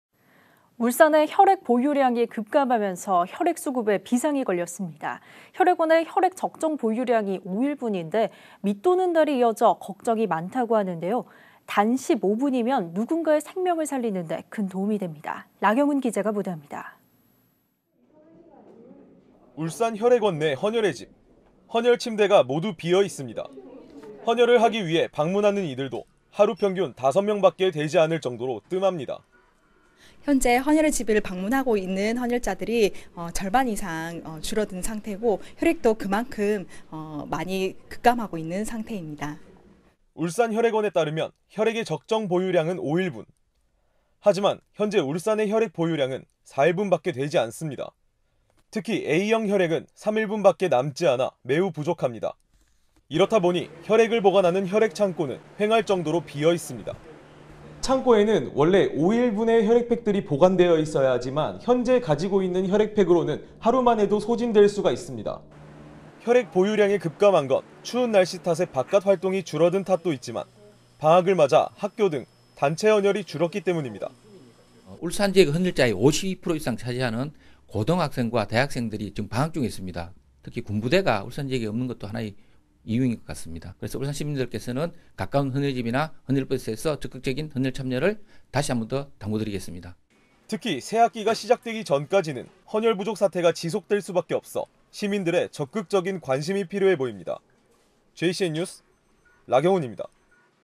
“15분이면 사람 살려”..헌혈 동참 시급
(앵커)
울산의 혈액 보유량이 급감하면서
혈액 수급에 비상이 걸렸습니다.

헌혈을 하는데 걸리는 시간은 15분이면 되지만
헌혈하는 시민들이 줄어들면서 보유량이 너무 적어
걱정이라고 합니다.

라경훈 기자가 보도합니다.

(기자)
울산 혈액원 내 헌혈의집.

헌혈 침대가 모두 비어 있습니다.

헌혈을 하기 위해 방문하는 이들도
하루 평균 5명밖에 되지 않을 정도로 뜸합니다.

(인터뷰) 김지윤 / 간호사
“현재 헌혈의 집을 방문하고 있는
헌혈자들이 절반 이상 줄어든 상태고
 혈액도 그만큼 많이 급감하고 있는 상태입니다.”

(CG IN) 울산혈액원에 따르면
혈액의 적정 보유량은 5일분.

하지만 현재 울산의 혈액 보유량은
4일분밖에 되지 않습니다.

특히 A형 혈액은
3일분밖에 남지 않아 매우 부족합니다. (CG OUT)

이렇다 보니 혈액을 보관하는
혈액 창고는 휑할 정도로 비었습니다.

(스탠드 업) 창고에는 5일분의 혈액팩들이 보관돼 있어야 하지만
지금 보유한 혈액팩으로는 하루 만에도 다 소진될 수 있습니다.

혈액 보유량이 급감한 건
추운 날씨 탓에 바깥 활동이 줄어든 탓도 있지만
방학을 맞아 학교 등
단체 헌혈이 줄었기 때문입니다.

(인터뷰) 최인규 울산혈액원장
“울산 지역 헌혈자의 52% 이상 차지하는
고등학생과 대학생들이 지금 방학 중에 있습니다.
특히 군부대가 울산 지역에 없는 것도 하나의 이유인 것 같습니다.
울산 시민들께서는 가까운 헌혈의 집이나
헌혈 버스에서 적극적인 헌혈 참여를 다시 한 번 더 당부 드리겠습니다.”

특히 새학기가 시작되기 전까지는
헌혈 부족 사태가 지속될 수밖에 없어
시민들의 적극적인 관심이 필요해 보입니다.

JCN뉴스, 라경훈입니다.//